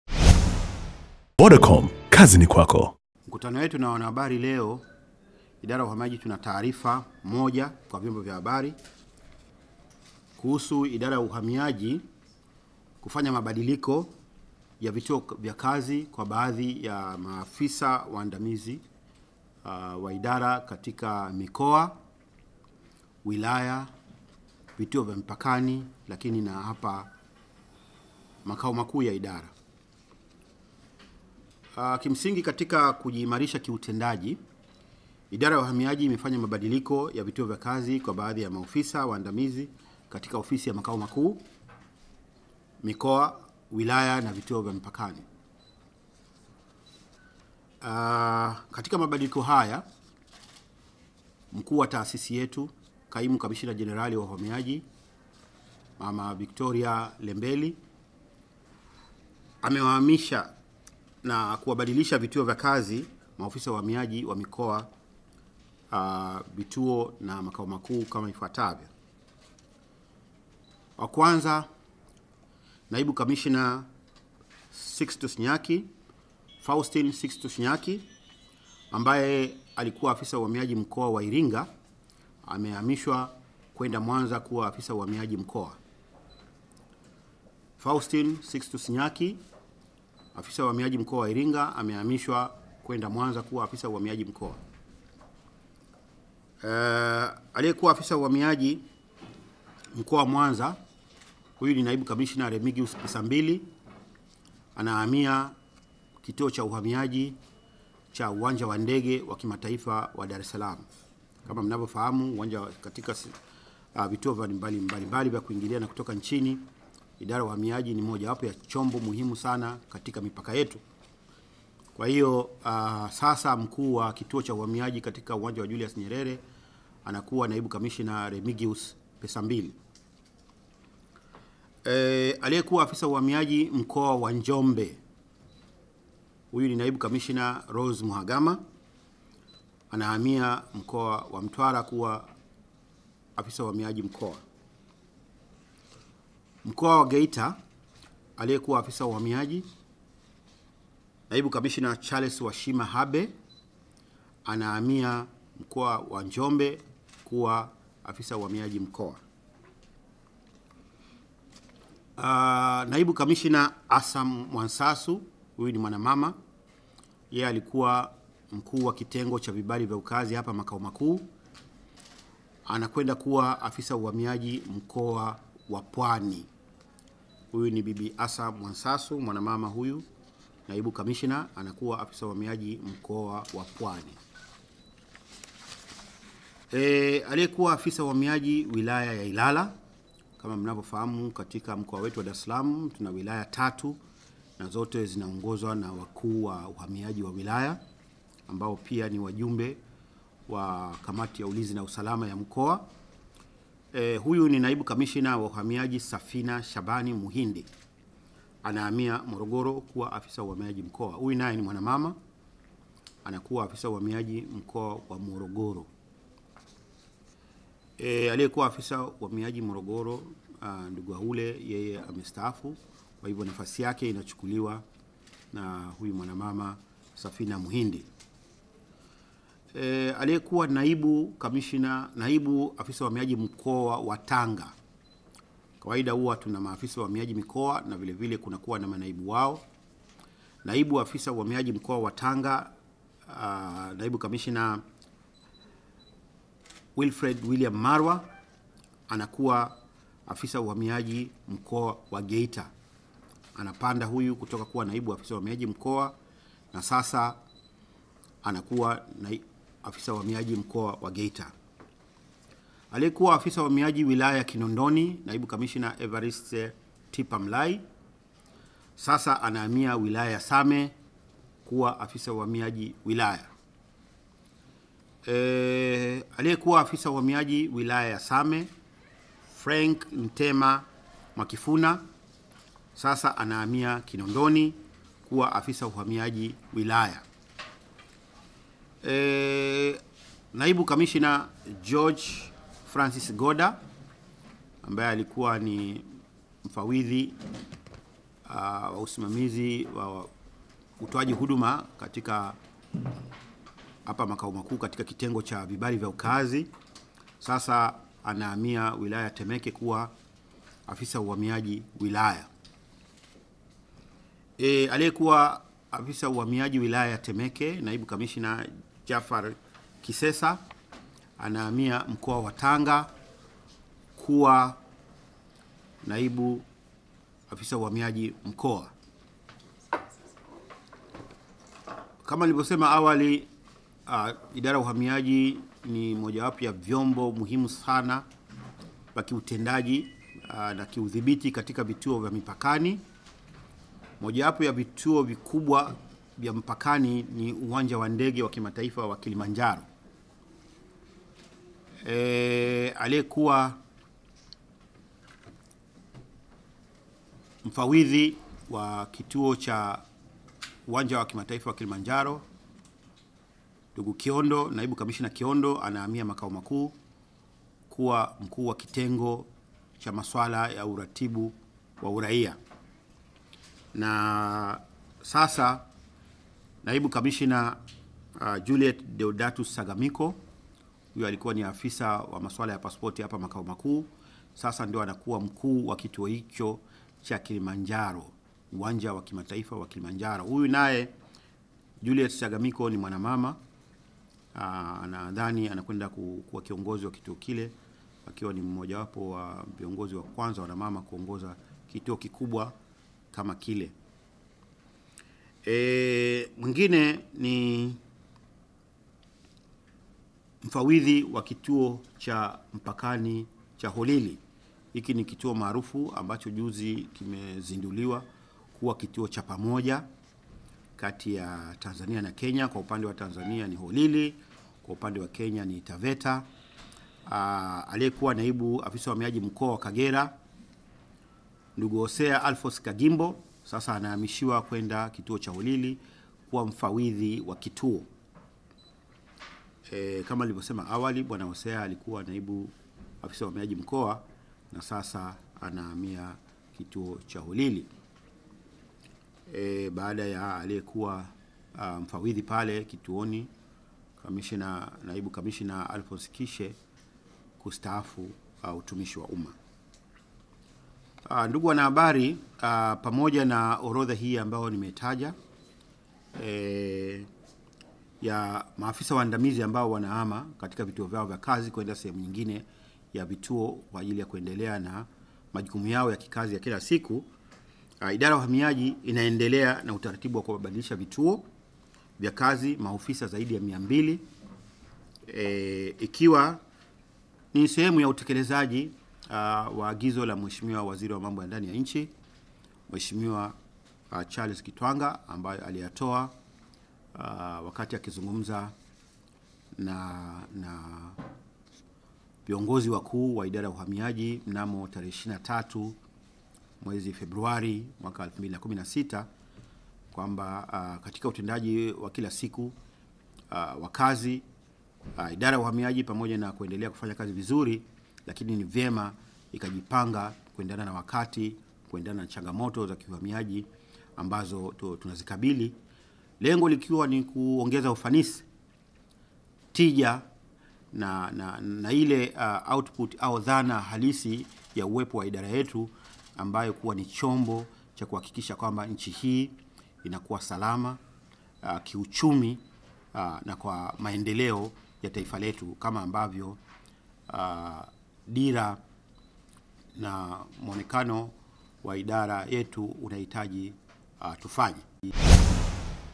Yaliyonifikia kutoka Wizara ya Mambo ya Ndani kitengo cha Uhamiaji
Wizara mbalimbali za Serikali hii ya awamu ya tano zinazidi kuingia kwenye headline, ikiwa ni siku kadhaa zimepita baada ya Waziri wa Mambo ya Ndani ya Nchi Charles Kitwanga kuagiza idara ya uhamiaji ifanyiwe mabadiliko makubwa ili kuboresha huduma na kuimarisha uadilifu katika vitengo vya idara hiyo nyeti.